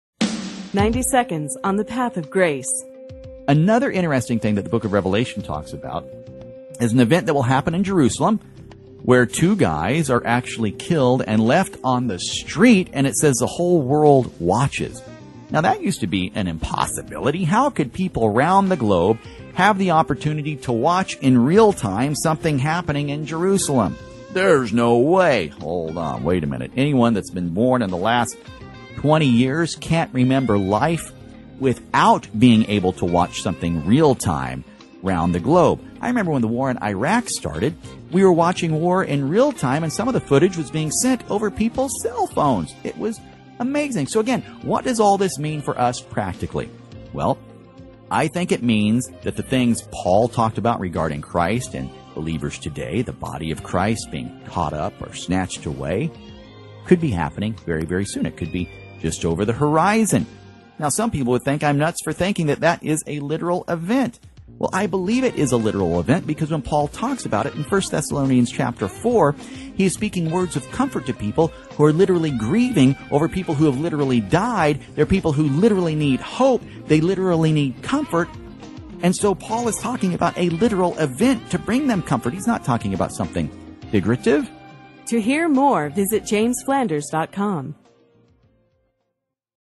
You Really Believe That Will Happen? - James Flanders
Another interesting thing that the book of Revelation talks about is an event that will happen in Jerusalem, where two guys are actually killed and left lying on the street and it says that the whole world watches.

That used to be an impossibility. How could people round the globe have the opportunity to watch in real-time something happening in Jerusalem? "There's no way!"

Hold on. Wait a minute. Anyone that has been born within the last twenty years can't remember life without being able to watch something real-time around the globe.

I remember when the war in Iraq started we were watching war in real-time and some of the footage was being sent over peoples cellphones. It was amazing!

So what does this all mean for us practically? Well, I think it means that the things Paul talked about regarding Christ and believers today... the Body of Christ being caught up or snatched away could be happening very very soon. It could be just over the horizon.

Some people would think I am nuts for believing that is a literal event. Well, I believe it is a literal event because when Paul talks about it in 1st Thessalonians chapter 4, he is speaking words of comfort to people who are literally grieving over people who have literally died. They are people who literally need hope. They literally need comfort and so Paul is talking about a literal event to bring them comfort. He's not talking about something figurative.

James Flanders